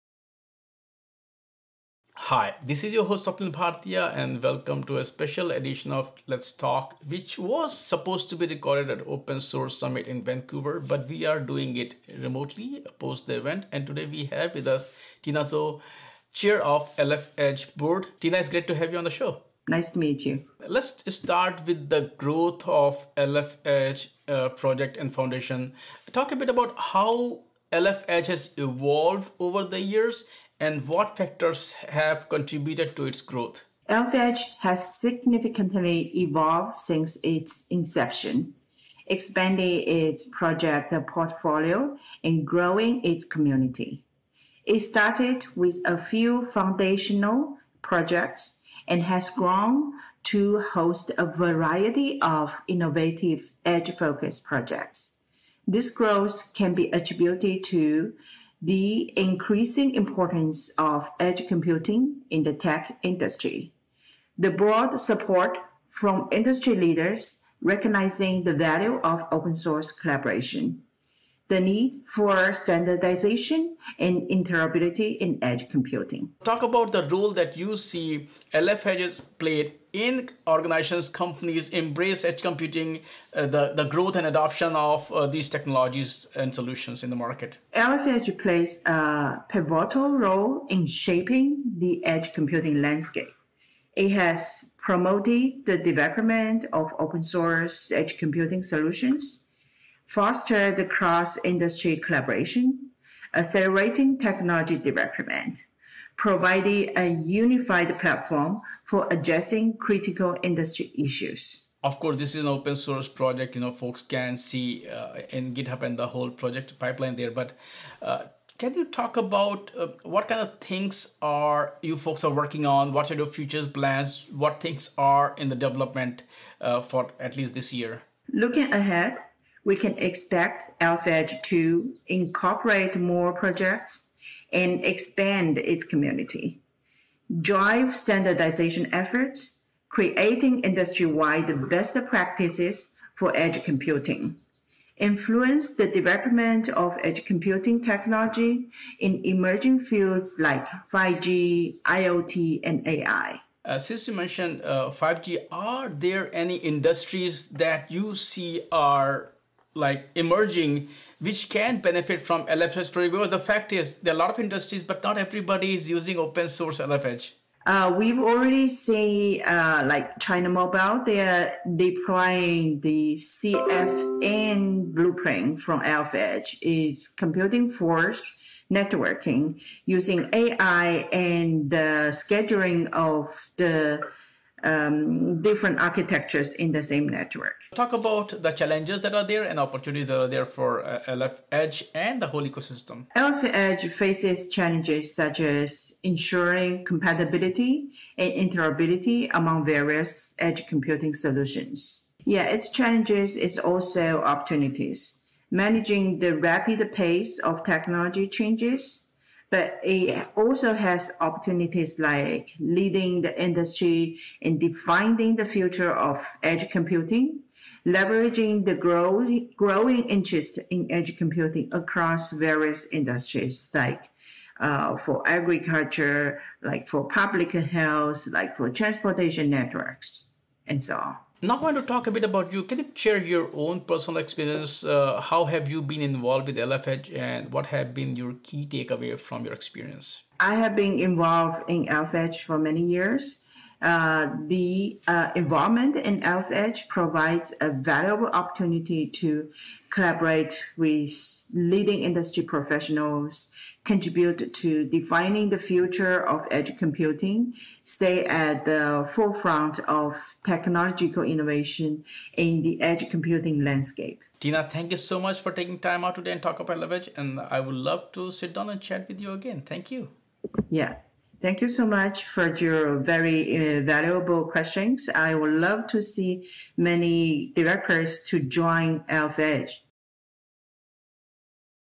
Adoption Of LF Edge Is Growing Across Industries : Tina Tsou
In this interview Tina Tsou, Governing Board Chair of LF Edge project gives us an update on the project and its adoption.